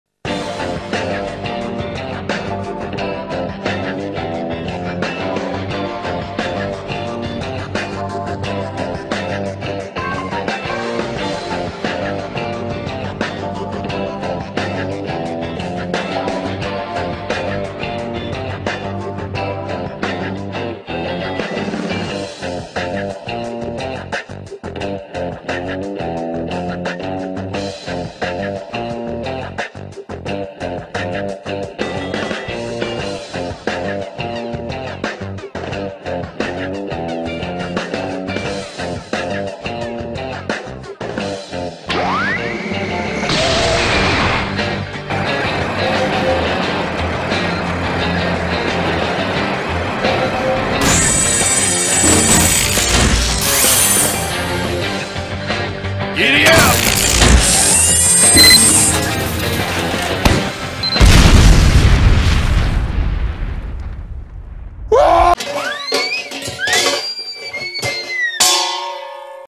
Test animation compilation
To prove I'm not dead, here's a collection of test animations I've made recently. Your boy's not done yet, just really busy so I can't make videos as often anymore. don't you just love having a job and a lack of motivation?